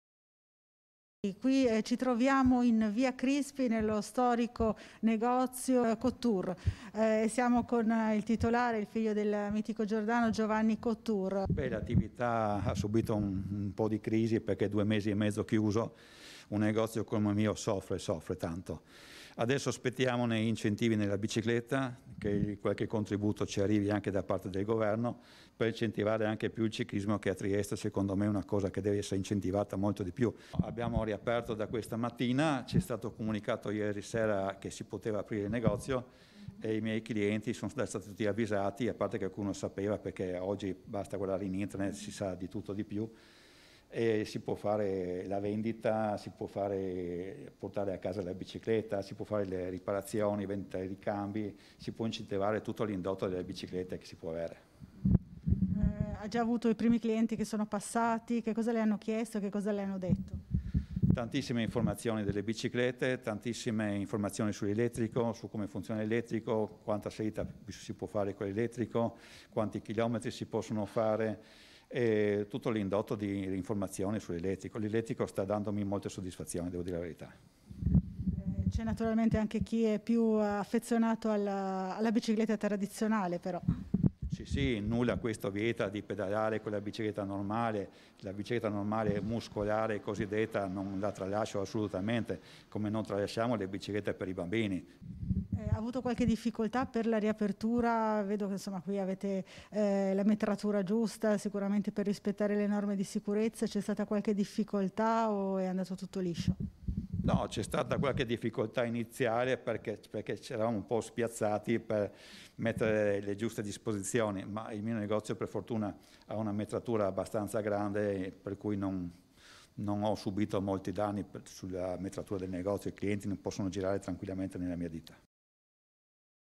06/05/2020 - FASE 2 RIAPRONO ANCHE I NEGOZI DI BICICLETTE
IL NOTIZIARIO (mercoledì 6 maggio 2020) - Tra i negozi che hanno potuto riaprire in questa fase ci sono anche quelli di biciclette e articoli per il ciclismo. Siamo andati in uno degli esercizi storici della nostra città, quello dell'indimenticato campione Giordano Cottur.